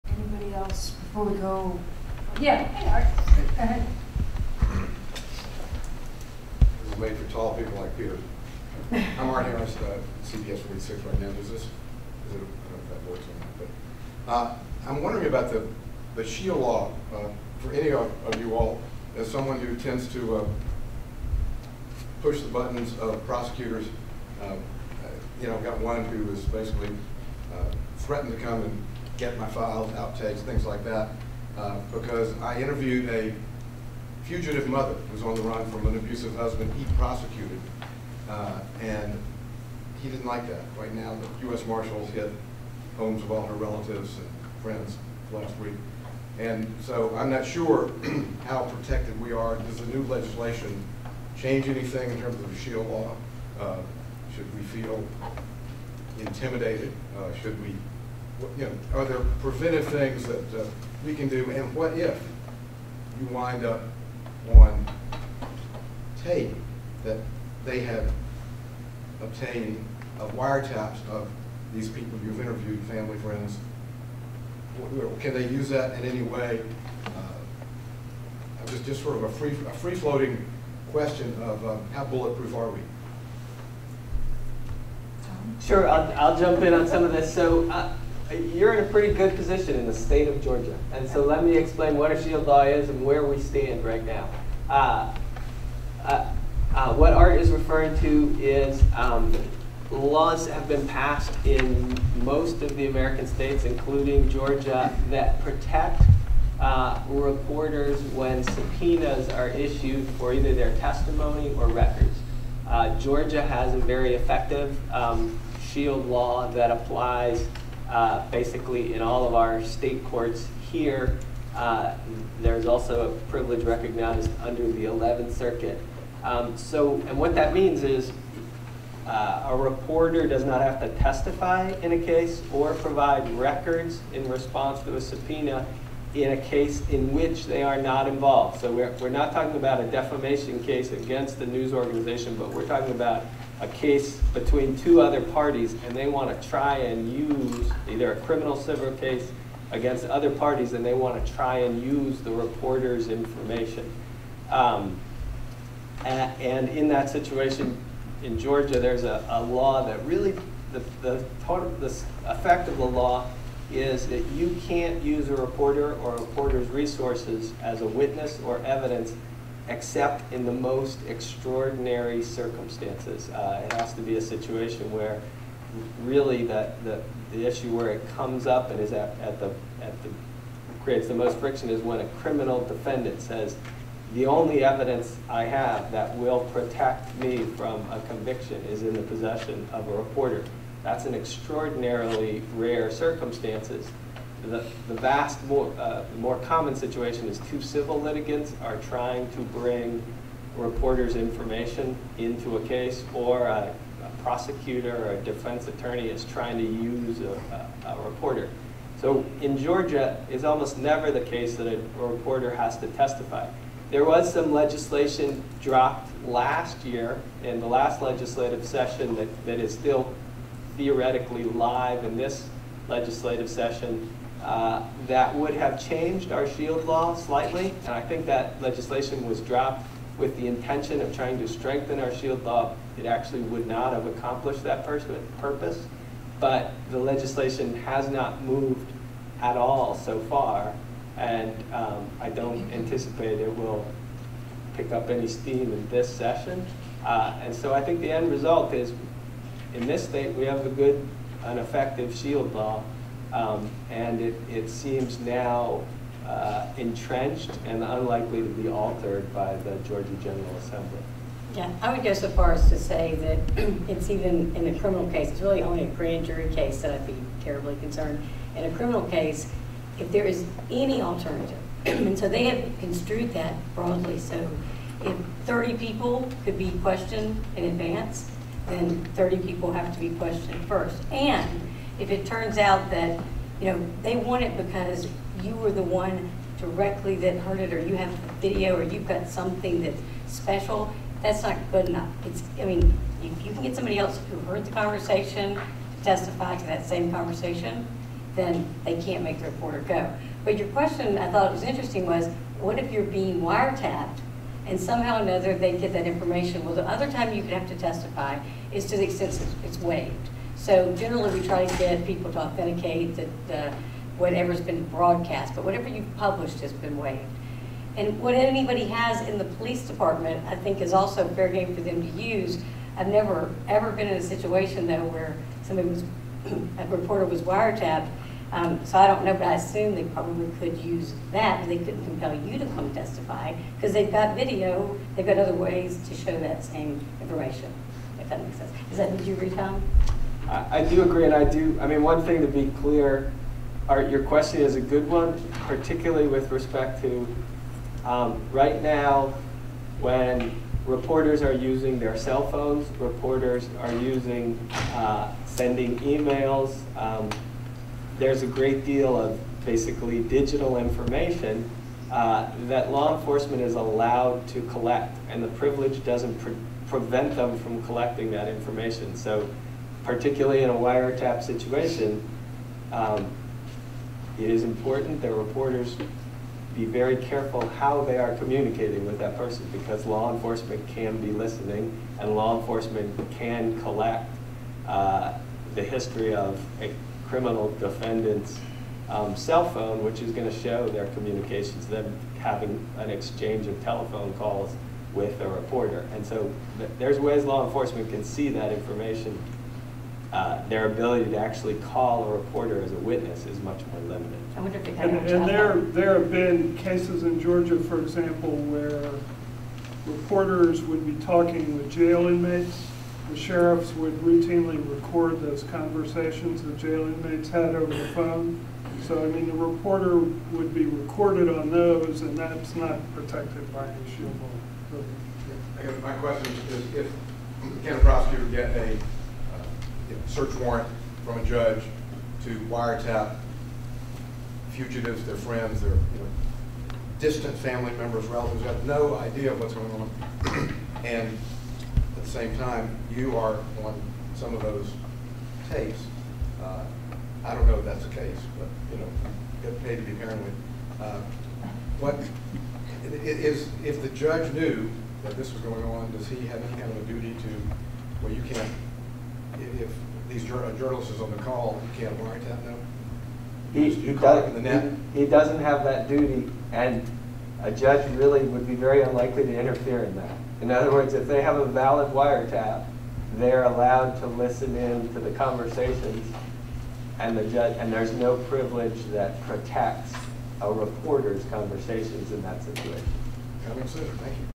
Art Harris on Shield Law for Reporters — Georgia First Amendment Foundation at WSB 01/28/16
CBS46 reporter Art Harris asks about shield law to protect reporters at the Georgia First Amendment Foundation legislative watch breakfast at WSB Studios in Atlanta, Georgia on Thursday, January 28, 2016.